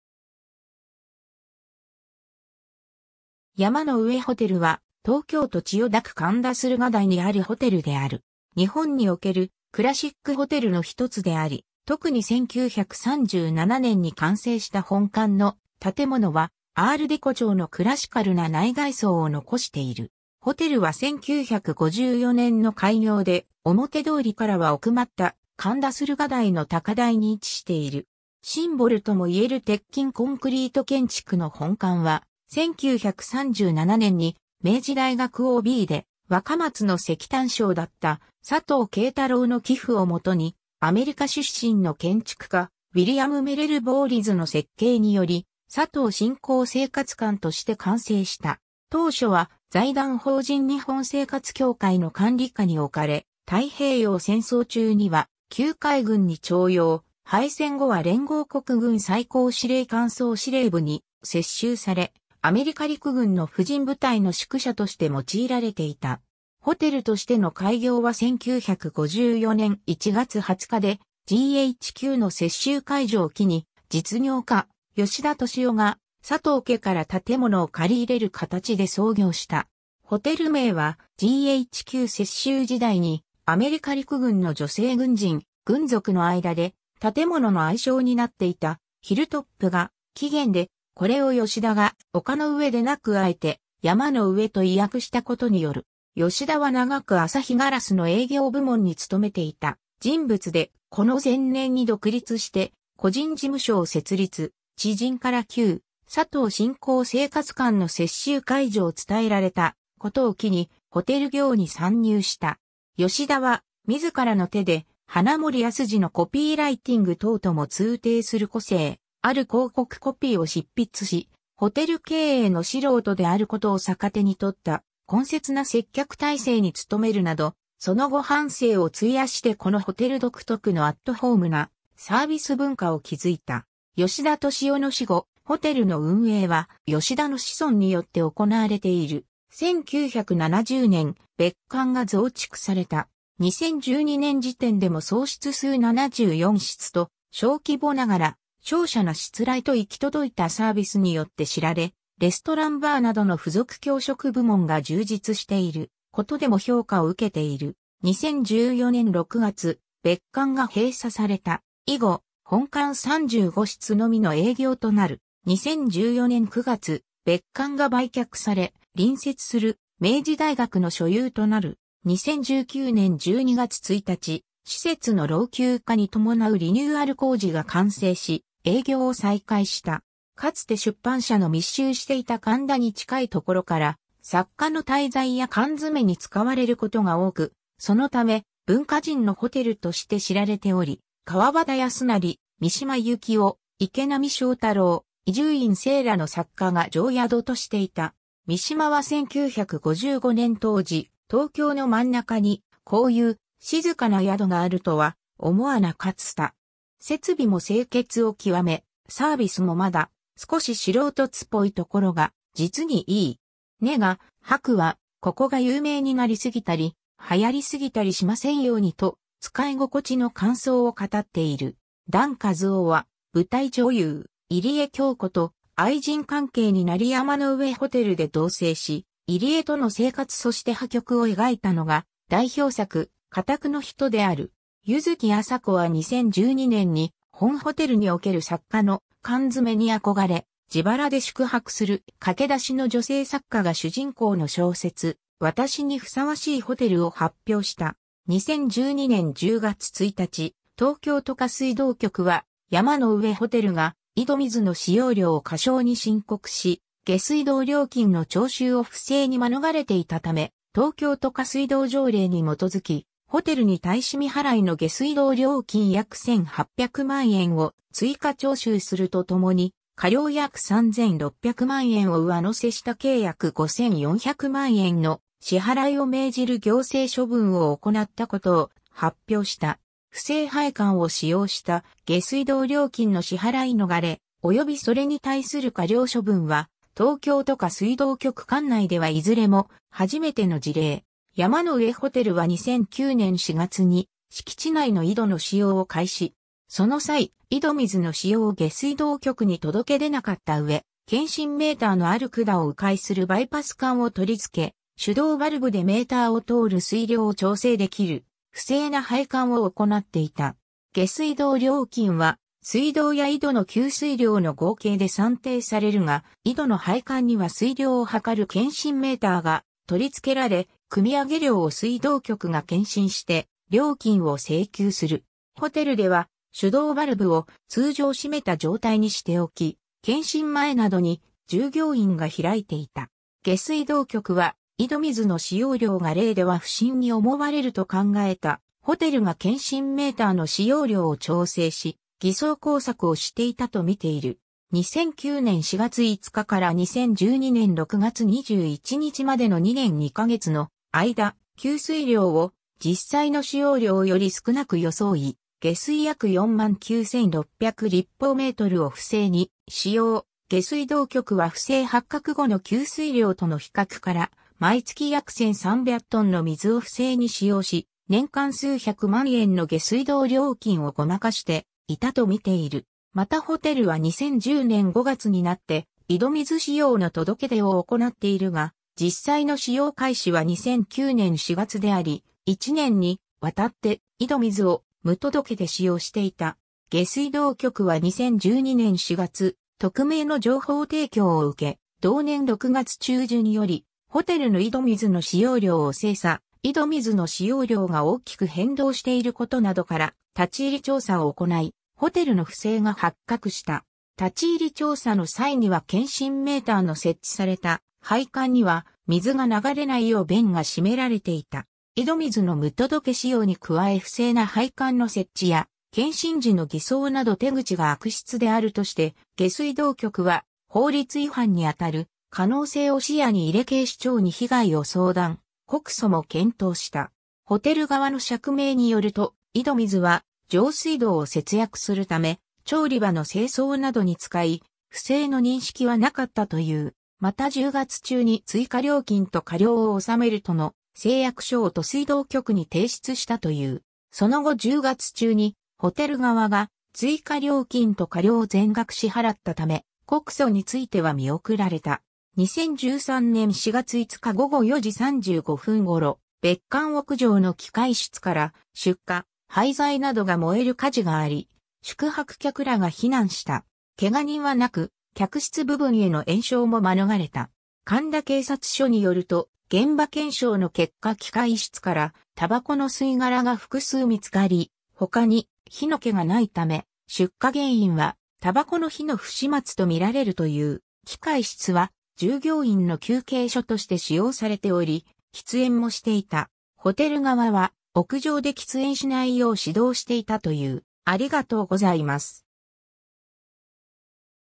山の上ホテル
ウィリアム・メレル・ヴォーリズ
アール・デコ建築
山の上ホテル（やまのうえホテル、Hilltop Hotel）は、東京都千代田区神田駿河台にあるホテルである。
日本におけるクラシックホテルのひとつであり、特に1937年（昭和12年）に完成した本館の建物はアール・デコ調のクラシカルな内外装を残している。
 ホテルは1954年（昭和29年）の開業で、表通りからは奥まった神田駿河台の高台に位置している。
 シンボルとも言える鉄筋コンクリート建築の本館は、1937年（昭和12年）に明治大学OBで若松（現在の北九州市）の石炭商だった佐藤慶太郎の寄付を基にアメリカ出身の建築家ウィリアム・メレル・ヴォーリズの設計により「佐藤新興生活館」として完成した。
 当初は財団法人日本生活協会の管理下に置かれ、太平洋戦争（大東亜戦争）中には旧海軍に徴用、敗戦後は連合国軍最高司令官総司令部（GHQ）に接収されアメリカ陸軍の婦人部隊の宿舎として用いられていた。
 ホテルとしての開業は1954年（昭和29年）1月20日で、GHQの接収解除を機に、実業家・吉田俊男（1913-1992）が佐藤家から建物を借り入れる形で創業した。
ホテル名は、GHQ接収時代にアメリカ陸軍の女性軍人・軍属の間で建物の愛称になっていた「Hilltop」が起源で、これを吉田が「丘の上」でなく敢えて「山の上」と意訳したことによる。
 吉田は長く旭硝子の営業部門につとめていた人物で、この前年に独立して個人事務所を設立、知人から旧・佐藤新興生活館の接収解除を伝えられたことを機にホテル業に参入した。
吉田は自らの手で花森安治のコピーライティング等とも通底する個性ある広告コピーを執筆し、ホテル経営の素人であることを逆手に取った懇切な接客体制に務めるなど、その後半生を費やしてこのホテル独特のアットホームなサービス文化を築いた。
吉田俊男の死後、ホテルの運営は吉田の子孫によって行われている。
 1970年、別館が増築された。
2012年（平成24年）時点でも総室数74室と小規模ながら、瀟洒なしつらえと行き届いたサービスによって知られ、レストラン・バーなどの付属供食部門が充実していることでも評価を受けている。
 2014年6月、別館が閉鎖された。
以後、本館35室のみの営業となる。
 2014年9月、別館が売却され、隣接する明治大学の所有となる。
 2019年12月1日、施設の老朽化に伴うリニューアル工事が完成し営業を再開した。
 かつて出版社の密集していた神田に近いところから、作家の滞在や缶詰（ここでは執筆促進目的の軟禁場所としてホテルに強制滞在させられること）に使われることが多く、そのため「文化人のホテル」として知られており、川端康成、三島由紀夫、池波正太郎、伊集院静らの作家が定宿としていた。
三島は1955年（昭和30年）当時、「東京の真中にかういふ静かな宿があるとは思はなかつた。設備も清潔を極め、サービスもまだ少し素人つぽい処が実にいい。ねがはくは、ここが有名になりすぎたり、はやりすぎたりしませんやうに」と使い心地の感想を語っている。
檀一雄は舞台女優・入江杏子と愛人関係になり山の上ホテルで同棲し、入江との生活そして破局を描いたのが代表作『火宅の人』である。
柚木麻子は2012年に、本ホテルにおける作家の「カンヅメ」に憧れ、自腹で宿泊する駆け出しの女性作家が主人公の小説『私にふさわしいホテル』を発表した。
 2012年10月1日、東京都下水道局は山の上ホテルが井戸水の使用量を過少に申告し、下水道料金の徴収を不正に免れていたため、東京都下水道条例に基づき、ホテルに対し未払いの下水道料金約1800万円を追加徴収するとともに、過料約3600万円を上乗せした計約5400万円の支払いを命じる行政処分を行ったことを発表した。
不正配管を使用した下水道料金の支払い逃れ、及びそれに対する過料処分は東京都下水道局管内ではいずれも初めての事例。
山の上ホテルは2009年（平成21年）4月に敷地内の井戸の使用を開始。
その際、井戸水の使用を下水道局に届け出なかった上、検針メーターのある管を迂回するバイパス管を取り付け、手動バルブでメーターを通る水量を調整できる不正な配管を行っていた。
下水道料金は水道や井戸の給水量の合計で算定されるが、井戸の配管には水量を測る検針メーターが取り付けられ、くみ上げ量を水道局が検針して料金を請求する。
ホテルでは手動バルブを通常...